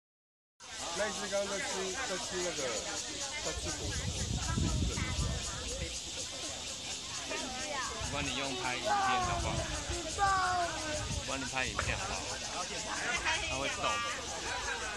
桃園 蘆竹【羊稠森林步道甲蟲季】獨角仙與各種大兜蟲 甲蟲生態遊覽 0707 269
桃園-蘆竹-羊稠坑森林步道擁有豐富的生態景觀；鳳尾蝶、松鼠、鍬形蟲、獨角仙穿梭在筆筒樹、油桐與相思樹間，展現多采多姿自然樣貌。
甲蟲季與羊稠坑步道影音圖文🔻

每年六到八月間是甲蟲孵育季，當地有大量獨角仙，還有機會遇到戰神大兜蟲、毛大象大兜蟲、長戟大兜蟲等外來種甲蟲。

羊稠步道四周動植物生態豐富，里辦公處也培訓環保志工、巡守隊導覽，向遊客介紹甲蟲生長過程與品種等知識，也有學校安排學生來此生態教學。

市府農業局在蘆竹區羊稠步道進行甲蟲復育，為持續推廣生態教育，農業局即日起至七月七日舉行「羊稠步道甲蟲季」活動;將地方生態和人文活動串連，發展成為羊稠坑社區的亮點。